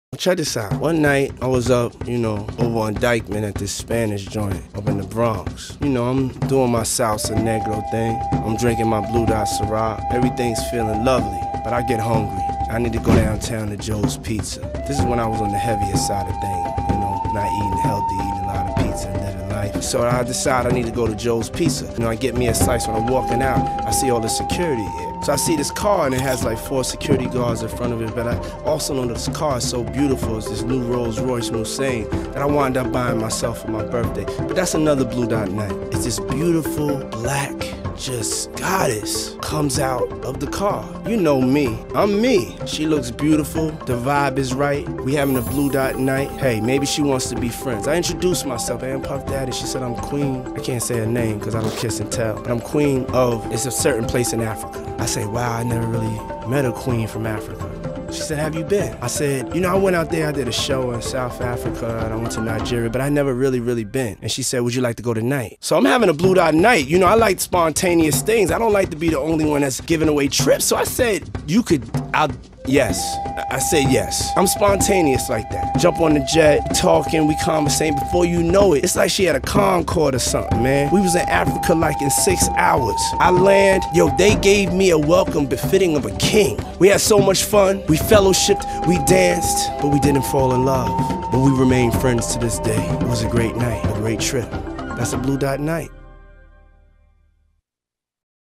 The Time I Met An African Queen - Blue Dot Nights
- I was out in New York having a Blue Dot Night and I randomly met an African Queen that offered to take me to Africa that night. Wow.